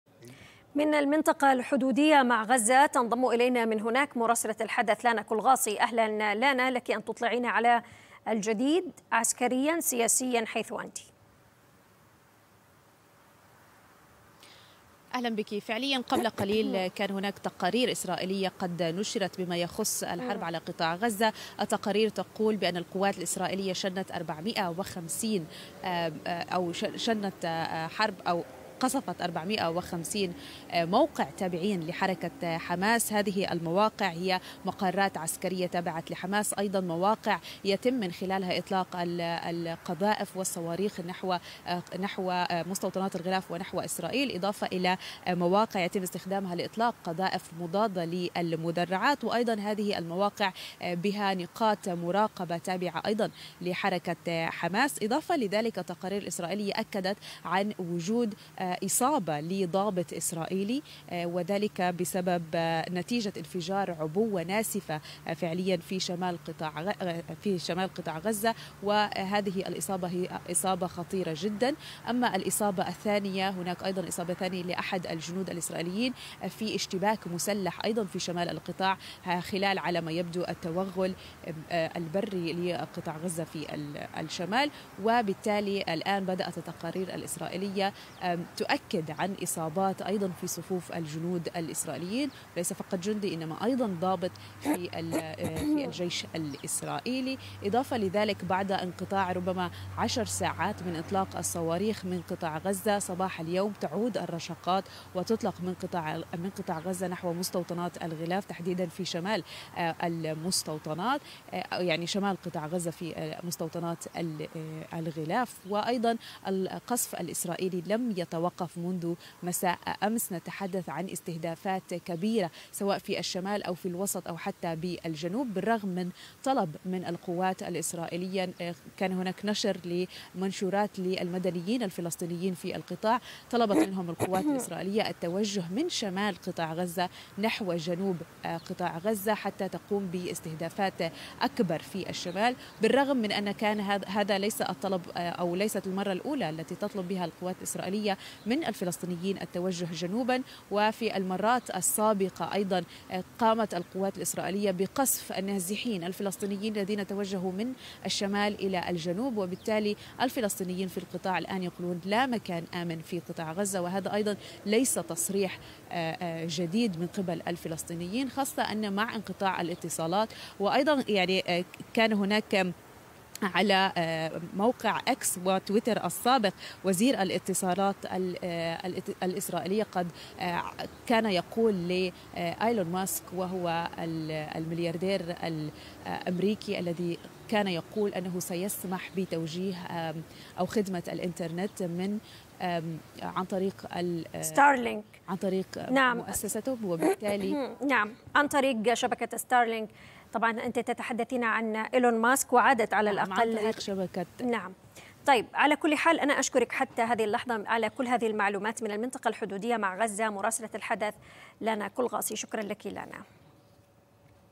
إصابة ضابط وجندي إسرائيلي بجروح خطيرة في مواجهات شمال غزة
مراسلة الحدث لانا كلغاصي: إصابة ضابط وجندي إسرائيلي بجروح خطيرة في مواجهات شمال قطاع غزة و إسرائيل تعلن مهاجمتها لـ 450 هدفا تابعا للفصائل الفلسطينية